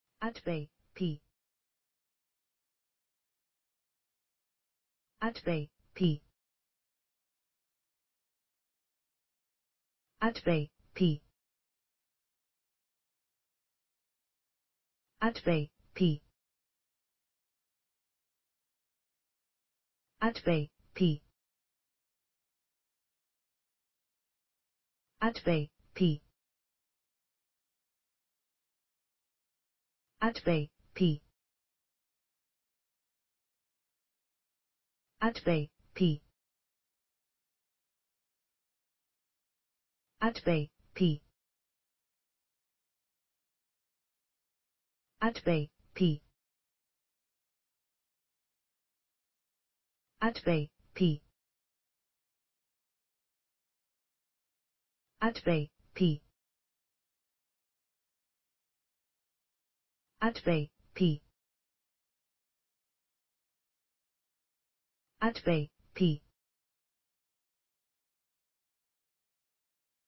at bay(p): How to pronounce at bay(p) in english (correct!).Start with A. Learn from me.
Follow me!Learn from me how to pronounce the word at bay(p) in english.This video shows you how to pronounce the word at bay(p) in english.

at bay(p)

adj.                forced to turn and face attackers   同义词：        cornered                         trapped                         treed